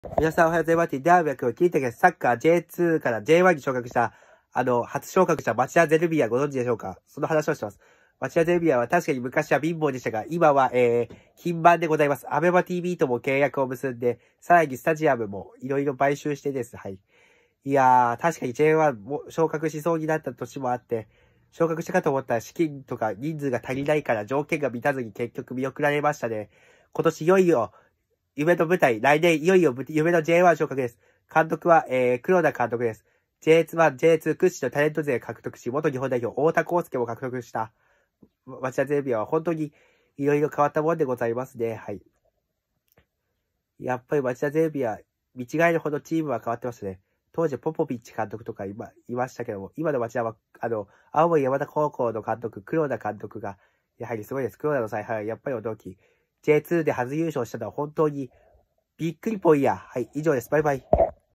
キングたくのYJ#21658
J1初昇格とJ2初優勝した町田ゼルビアの話。